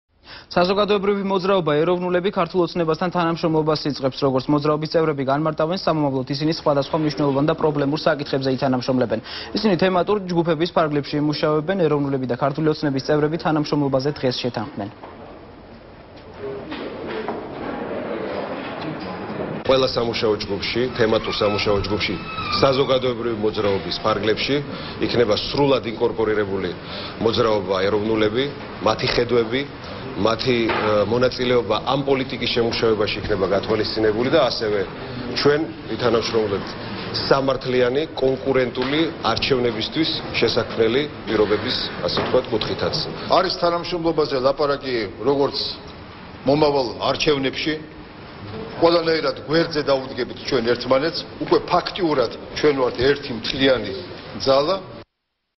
ახალი 6 | "ეროვნულები" " ოცნებაში" | 18.07.12
საზოგადოებრივი მოძრაობა ~ეროვნულები" „ქართულ ოცნებასთან" თანამშრომლობას იწყებენ. როგორც მოძრაობის წევრები განმარტავენ, სამომავლოდ ისინი სხვადასხვა მნიშვნელოვან და პრობლემურ საკითხებზე ითანამშრომლებენ. ისინი თემატური ჯგუფების ფარგლებში იმუშავებენ. ეროვნულები და ქართული ოცნების წევრები თანამშრომლობაზე  დღეს შეთანხმდნენ.